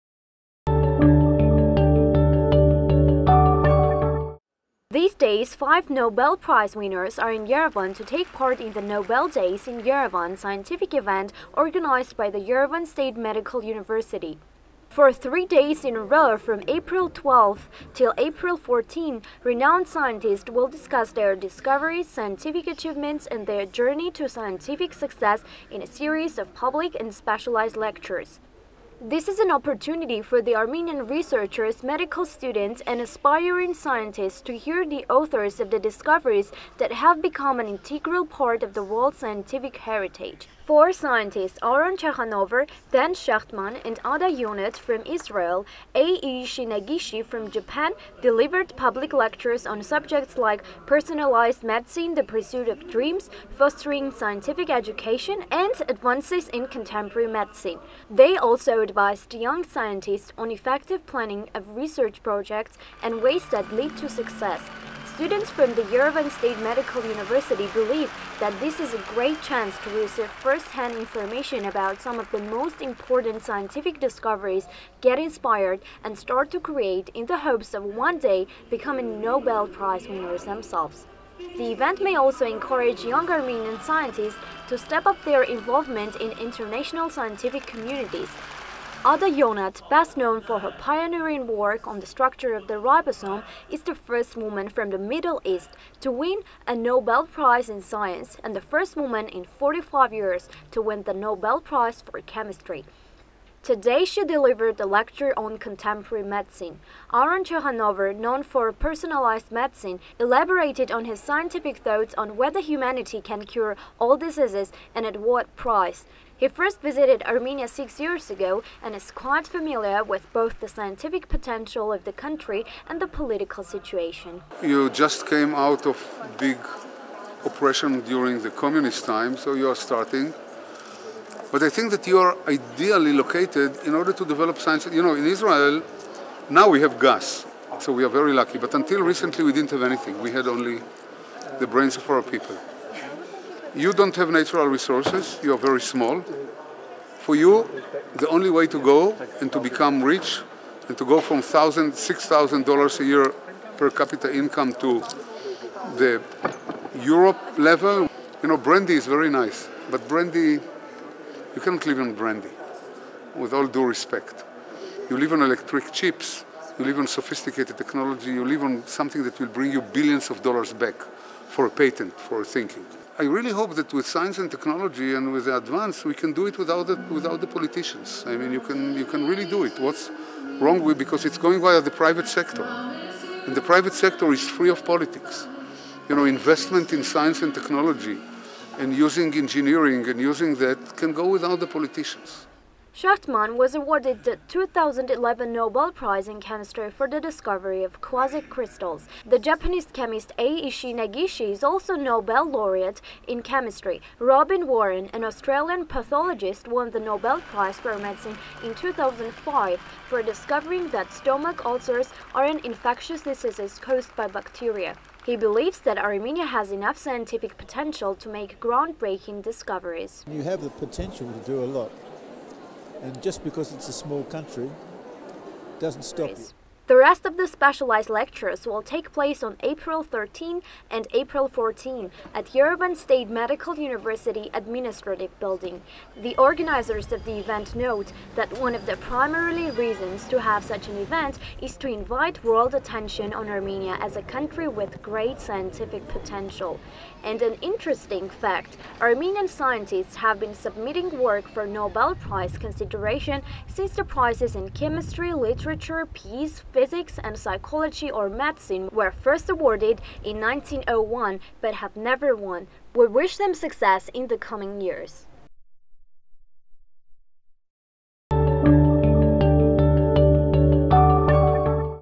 Five Scientists Under One Roof
Yerevan State Medical University after M. Heratsi organizes “Nobel Days in Yerevan” scientific event on 11-16 April 2016 and to simultaneously host five Nobel Laureates under one roof.